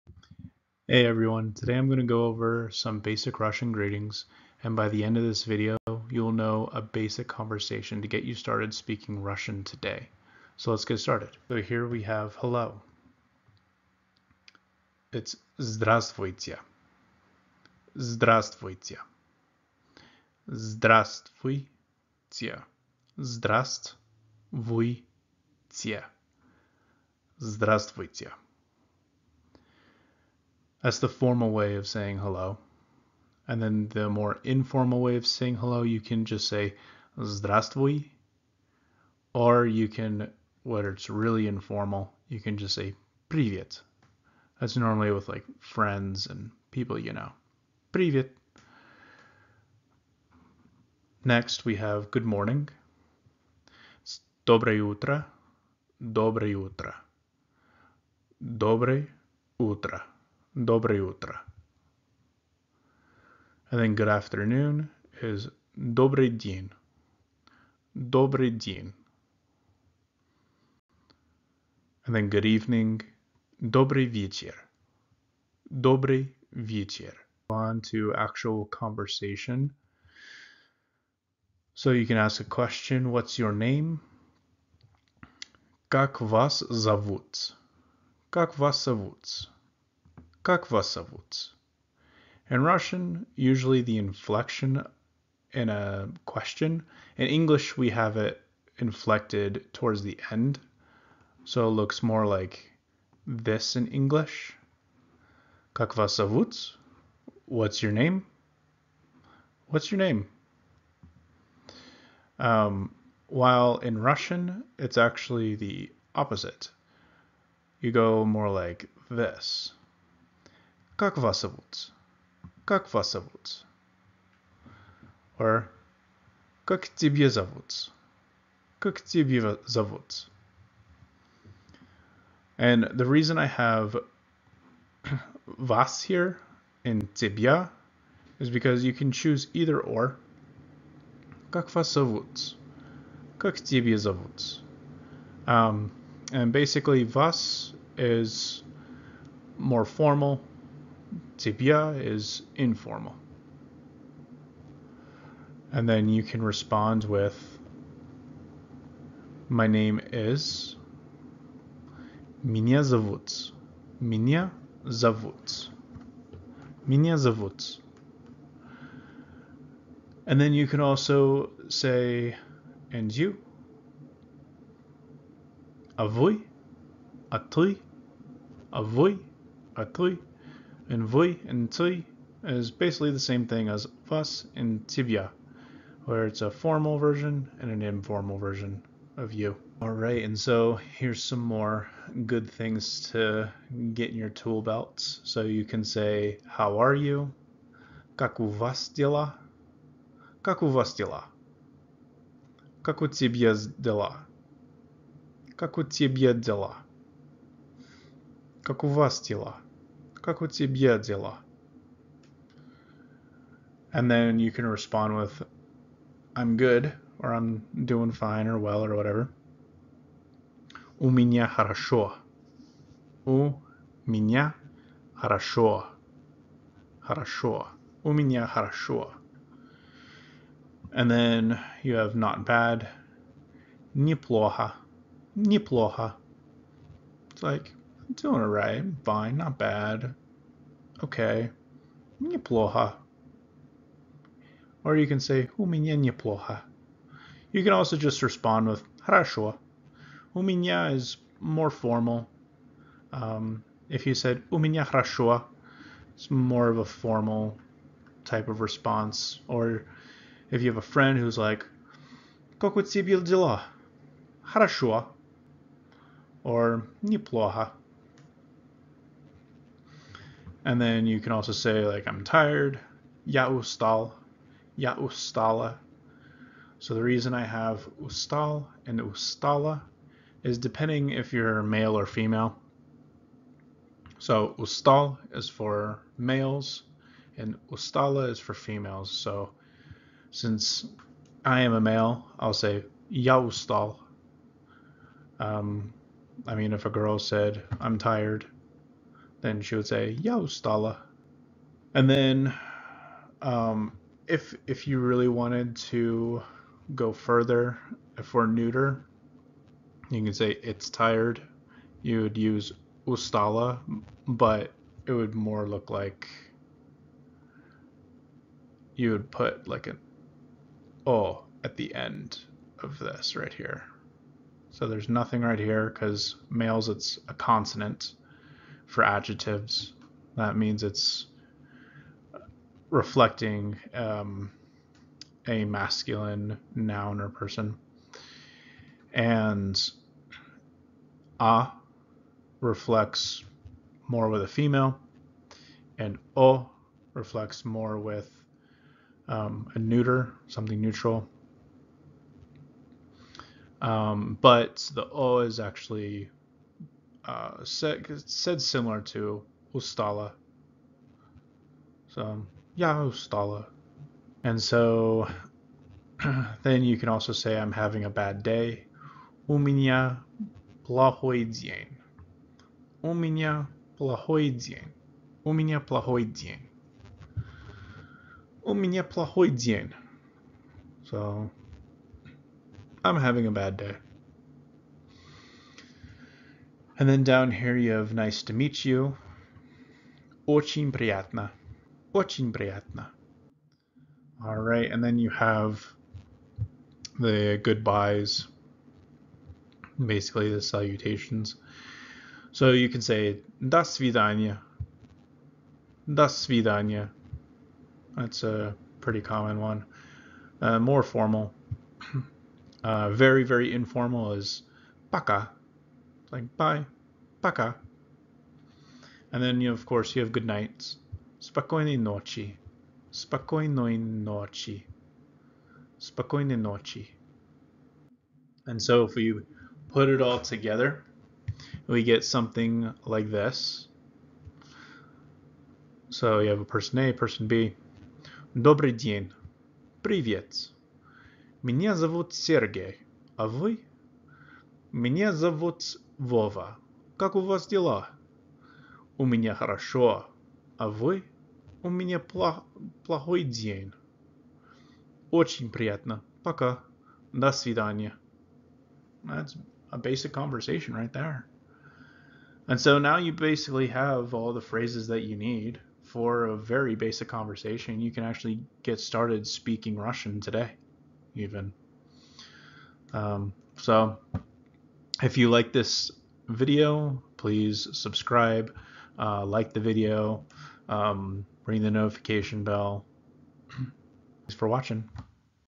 Russian Greetings for Beginners
I'm going over basic Russian greetings so that you can start speaking Russian today. These are the very basics and are a part of the Russian 100 level or A1 level courses on this channel.